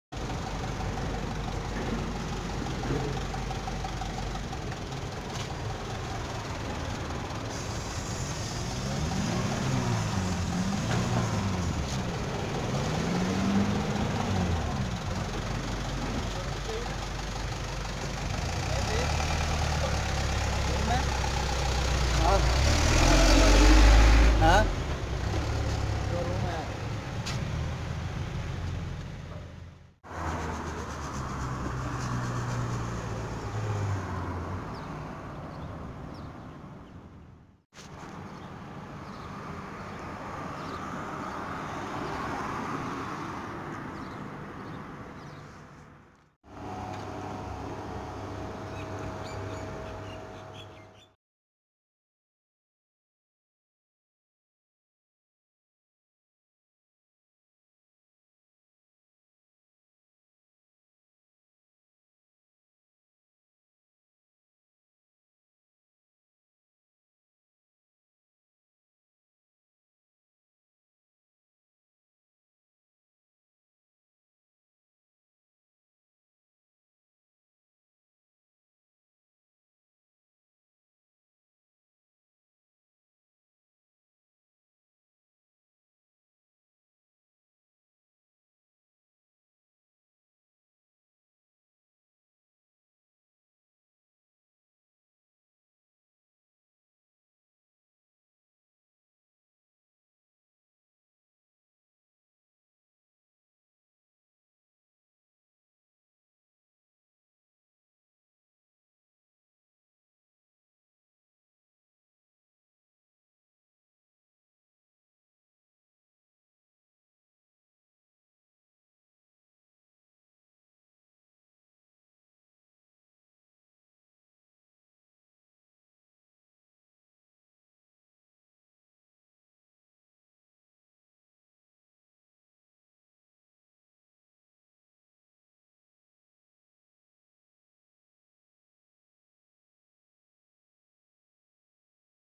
Looking for room in Qatar? Innovative places for ads!!
Looking for a bed space or a room for rent in Qatar then need not scan the newspapers or websites...supermarkets, grocery and building are places where you will find accommodation advertisements and also road side walls and even trees where they hung the space selling ads